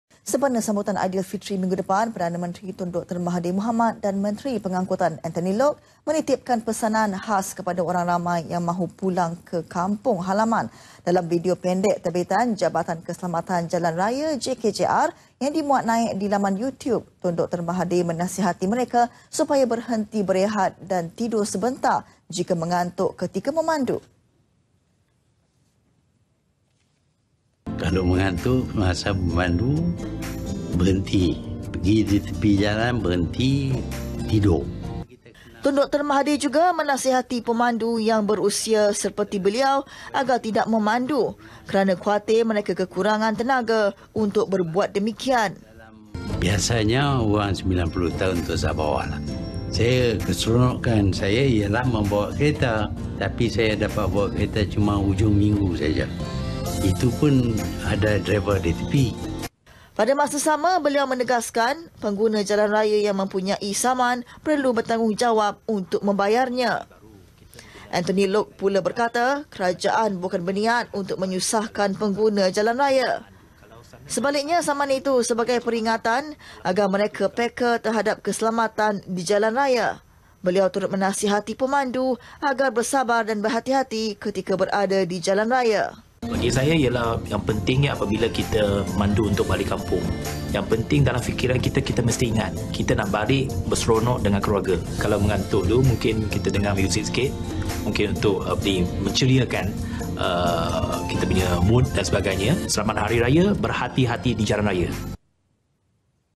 SEMPENA SAMBUTAN AIDILFITRI - PM & ANTHONY LOKE KONGSI PESANAN KESELAMATAN JALAN RAYA [10 JUN 2018]
Sempena sambutan Aidilfitri minggu depan, Perdana Menteri Tun Dr Mahathir Mohamad dan Menteri Pengangkutan Anthony Loke , menitipkan pesanan khas kepada orang ramai yang mahu pulang ke kampung halaman.

Dalam video pendek terbitan Jabatan Keselamatan Jalan Raya (JKJR),  yang dimuat naik di laman Youtube,  Tun Dr Mahathir menasihati mereka supaya berhenti berehat dan tidur sebentar,  jika mengantuk ketika memandu.